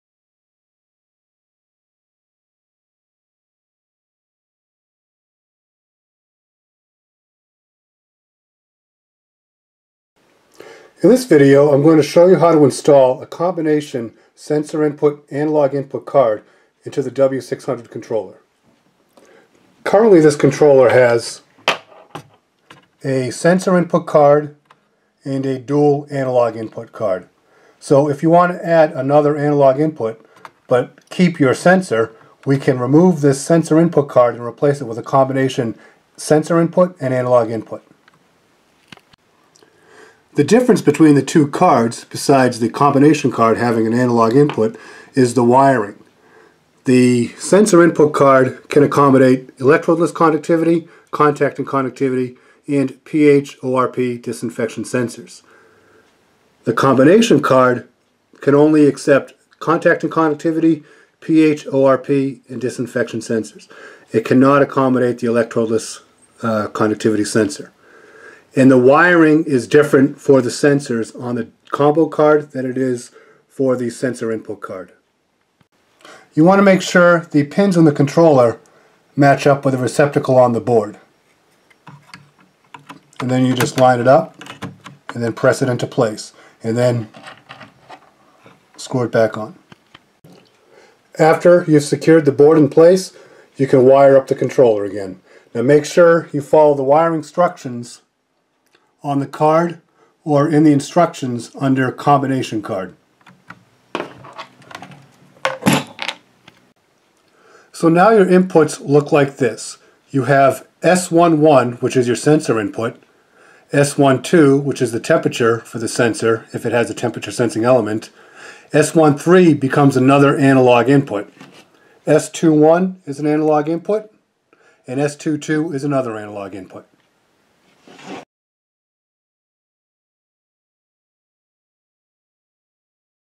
The W600: Installing the combonation sensor / analog input card
Installing and wiring tips for the Walchem combination analog/sensor input card